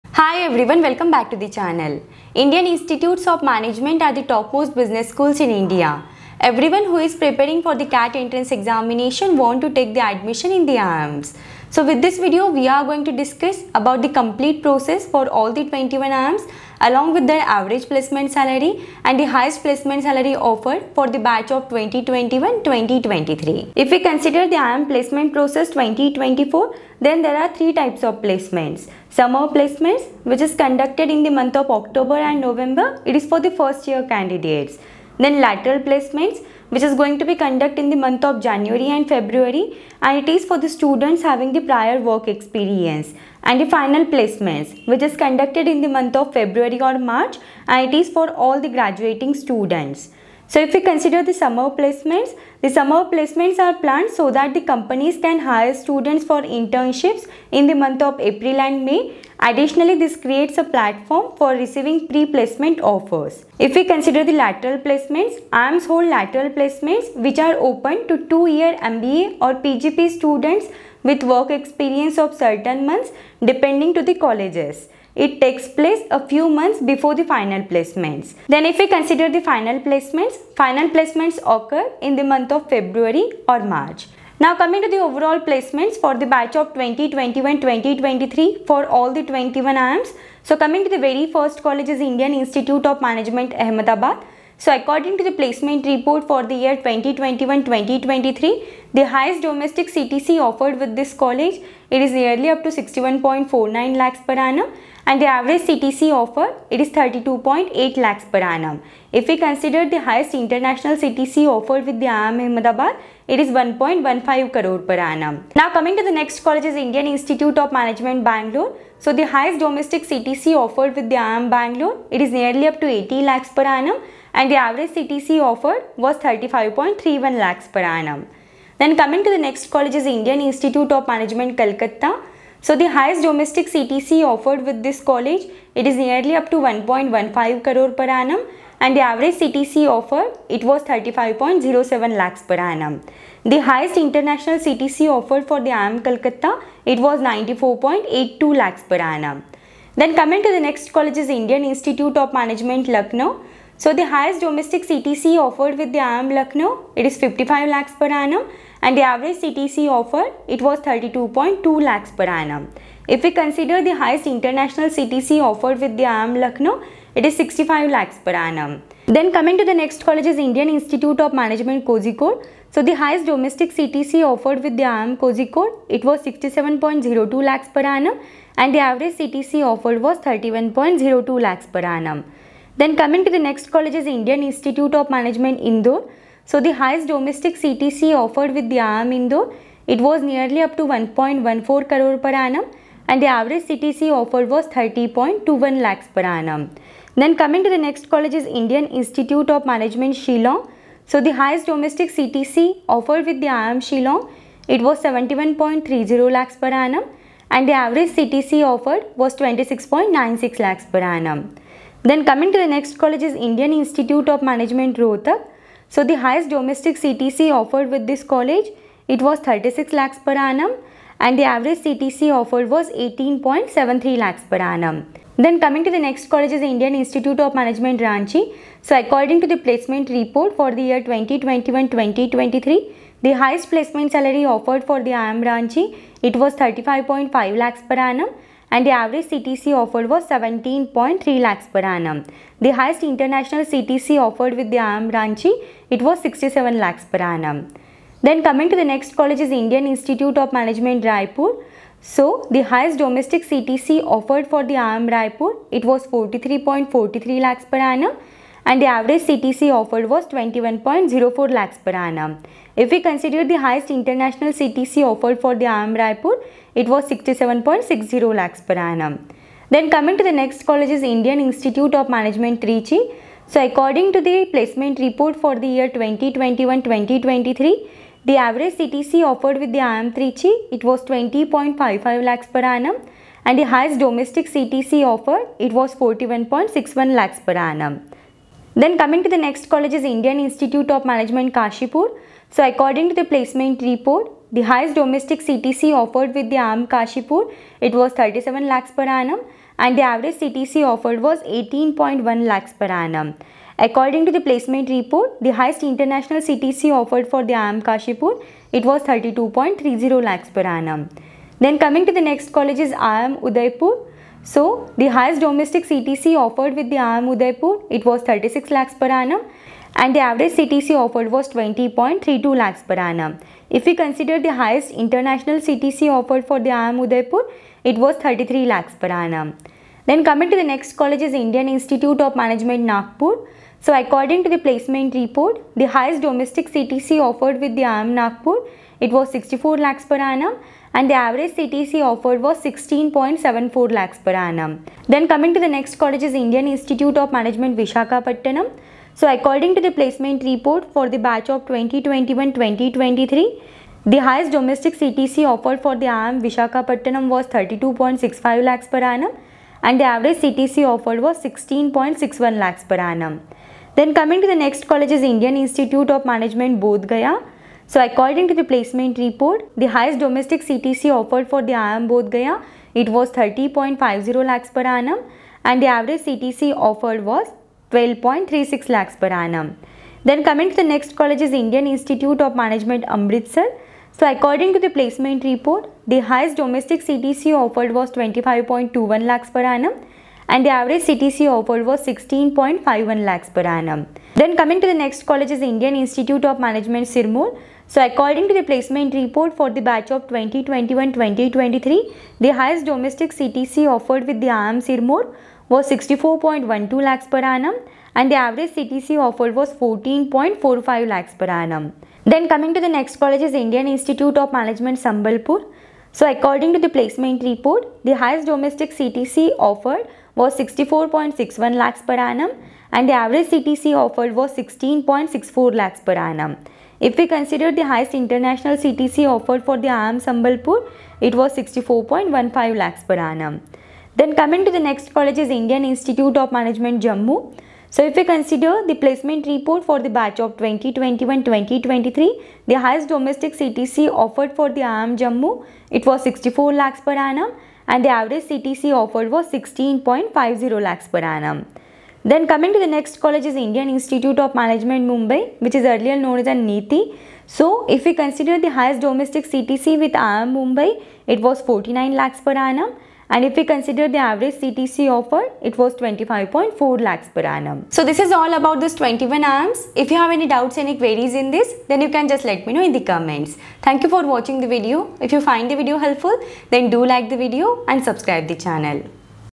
Placements for ALL IIMS🔥 Average & Highest Salary Package offered at 21 IIMS in India
IIM Ahmedabad Placement, IIM average CTC, IIM Highest CTC, IIM Placement, IIM Placements.